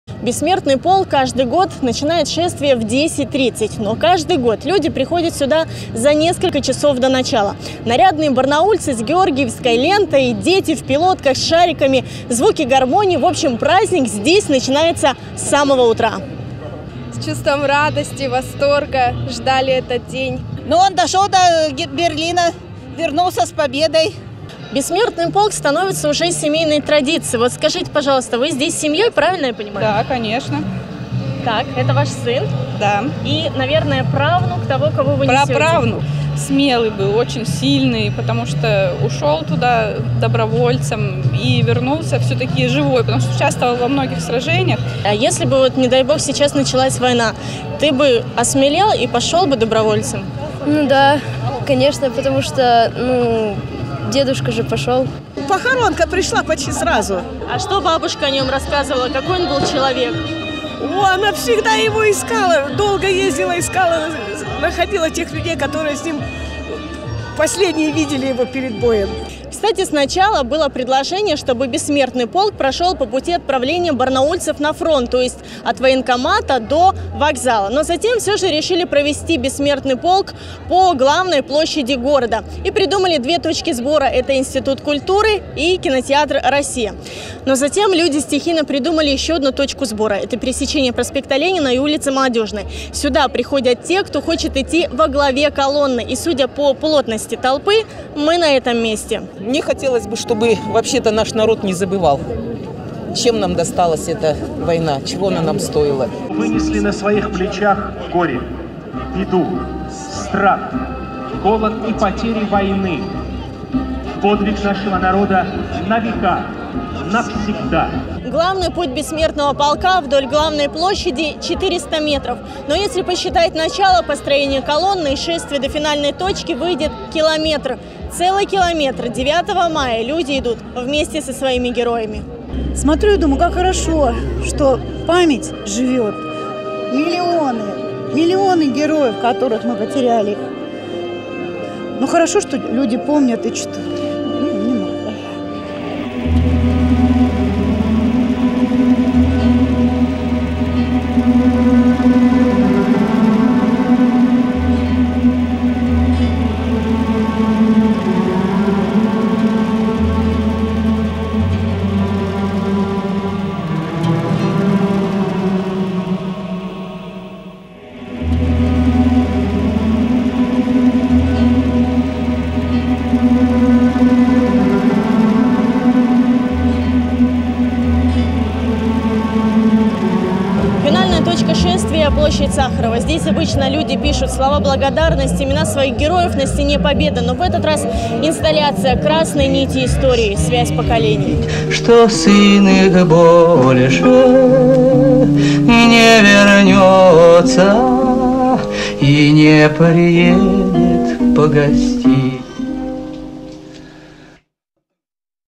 «Бессмертный полк»: в чем ценность и почему все больше людей присоединяются к акции?
На главную площадь каждого населенного пункта края сегодня вышли и сотни тысяч участников боевых действий – пусть и на портретах. Сегодня уже в седьмой раз на Алтае прошла победная акция «Бессмертный полк». В Барнауле в этом году в ней приняли участие почти пятьдесят тысяч человек. О ее ценности горожане рассказали Анастасии Дараган.